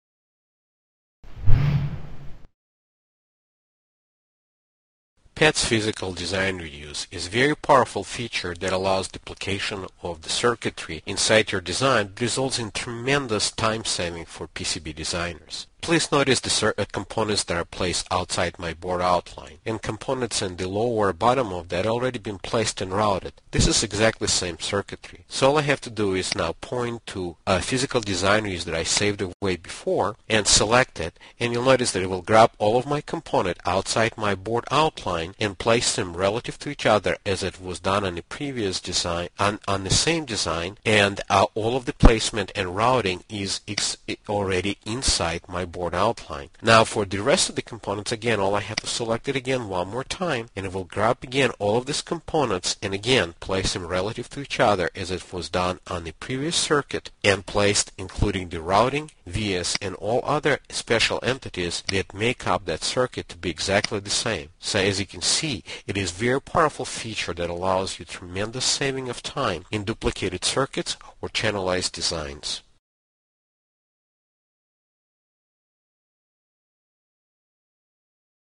PADS Demo - Physical Design Reuse
Reuse proven circuits and eliminate repetitive work with PADS physical design reuse. This short video shows you how PADS automatically picks unplaced components and arranges them in a predefined pattern, including pre-routed traces and vias connecting component pins.